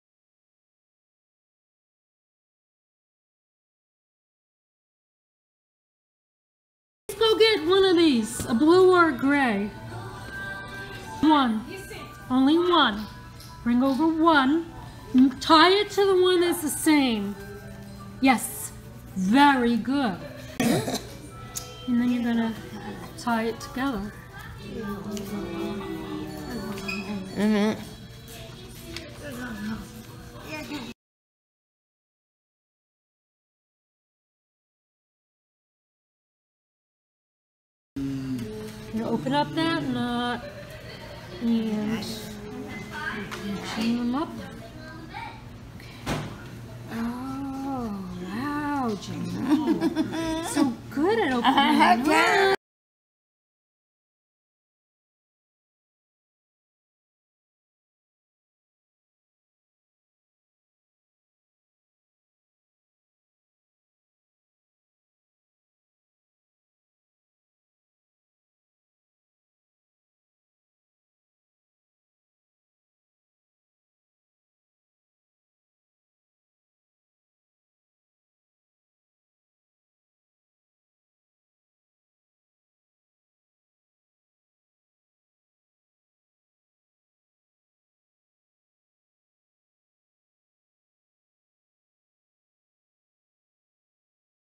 Tying or Untying Knots on Weighted Bags for Sensory Processing
Here is a fun way to teach how to tie or untie a knot. I inserted plastic bags of sand inside socks, sleeves or other fabric and tied the pink, fleece fabric strips to them. The fleece is thick and easier to manipulate than thin string.brbrI adapted the activity to require matching the colors or fabric designs on the bags. Carrying heavy bags and all the pulling provides sensory stimulation. The client in the video is quite good at tying and untying the knots.brbrAnother client enjoyed most of the steps to this task. However, he has poor motor planning skills and required hand-over-hand assistance to grasp and pull apart the fabric ends to tighten the loose knot that I made.brbrTeaching the last step of a task first is a technique called “backward chaining”. My client experiences success as he completes the easiest and final step in the sequence to knot tying.brbrLearn more about activity adaptations on my website and blog:bra href=" rel=nofollow target=_blank href=" rel=nofollow target=_blank